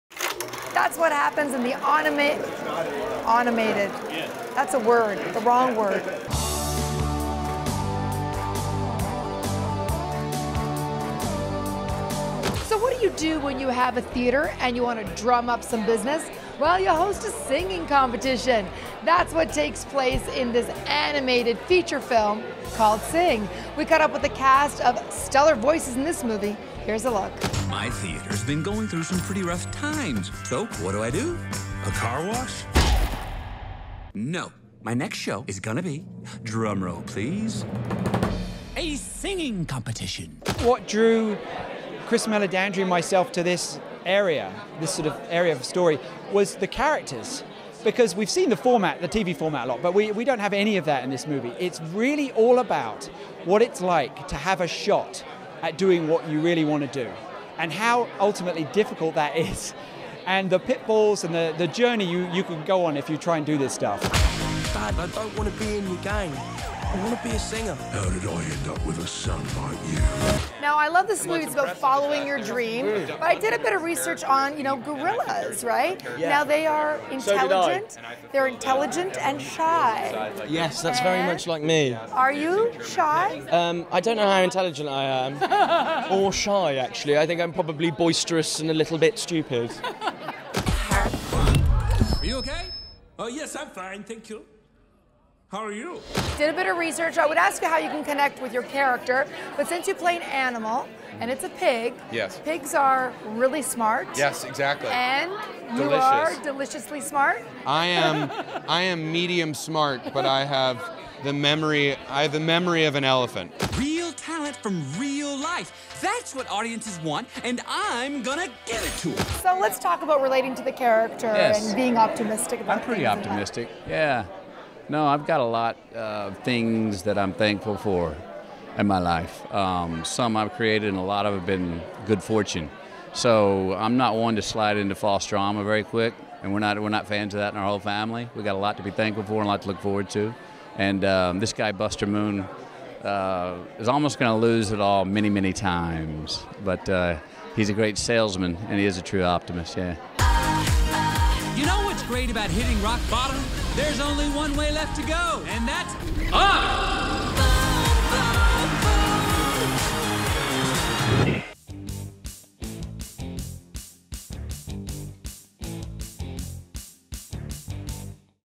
Matthew McConaughey and Nick Kroll - Sing
Melissa interviews the stars and creators of the film that sees animals chasing their singing dreams Sing! Check it out!